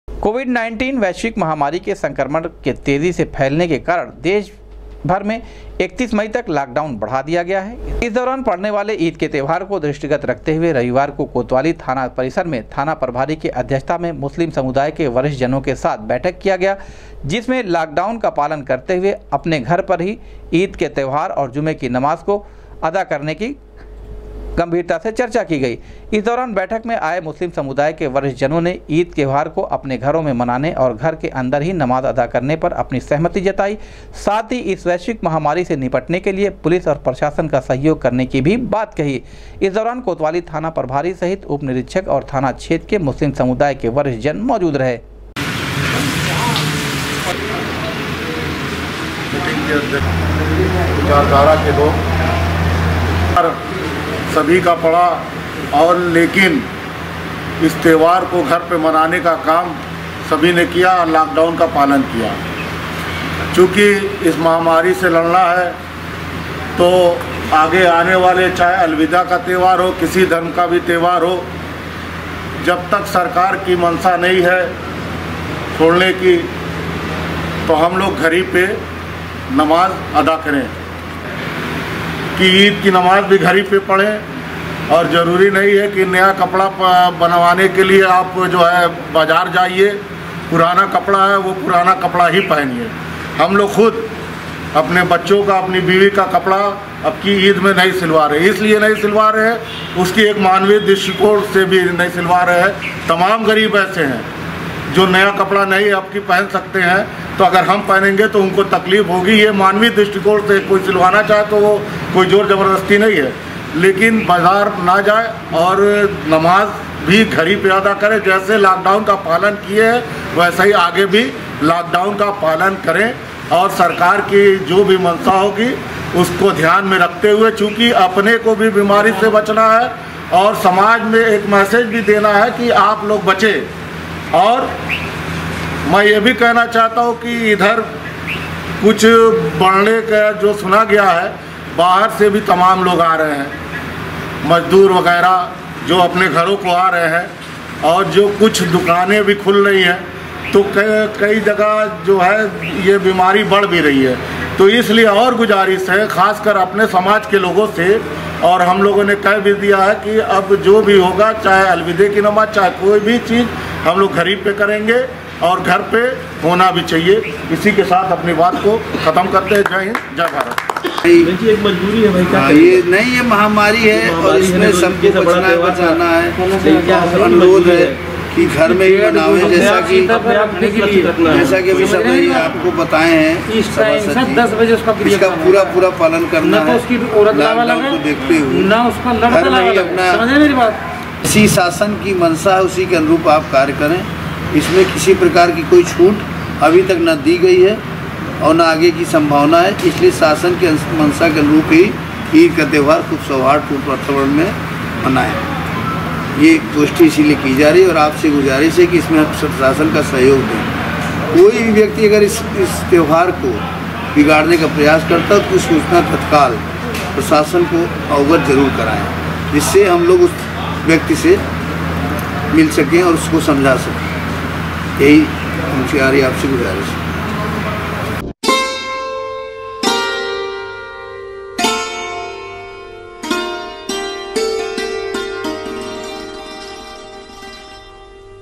कोतवाली थाना प्रभारी की अध्यक्षता में मुस्लिम समुदाय के वरिष्ठ जनों के साथ आयोजित की गई पीस कमेटी
कोतवाली थाना प्रभारी की अध्यक्षता में मुस्लिम समुदाय के वरिष्ठ जनों के साथ आयोजित की गई पीस कमेटी की बैठक, अलविदा जुमे की नमाज और ईद त्यौहार को अपने घरों में ही मनाने की बनी सहमति